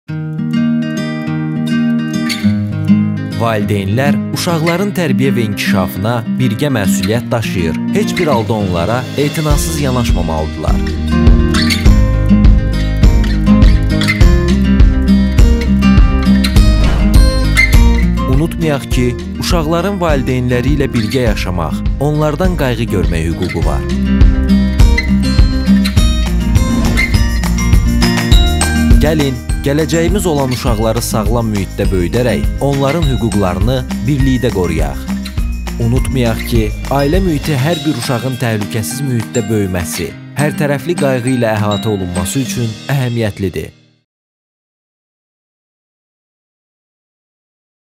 Hüquqlarını bilən uşaqlarla gələcəyə doğru!
Ailə, Qadın və Uşaq Problemləri üzrə Dövlət Komitəsi, Təhsil Nazirliyi və “Regional İnkişaf” İctimai Birliyinin birgə əməkdaşlığı çərçivəsində uşaq hüquqları mövzusunda hazırlanmış növbəti sosial çarxı təqdim edirik.